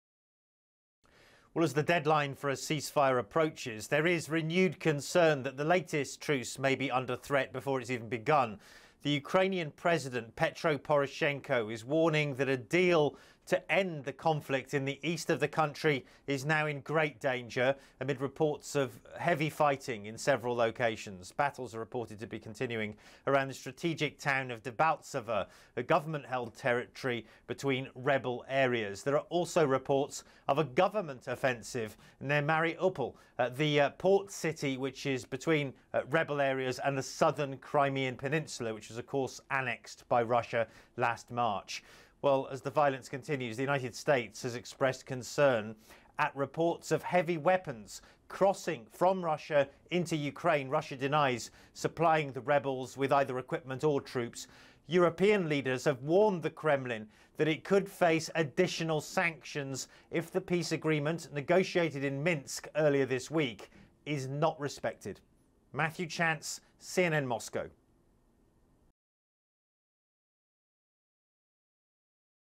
WILL UKRAINE CEASEFIRE HOLD?
The Ukraine ceasefire starts Sunday but questions remain if the agreement is solid enough to hold.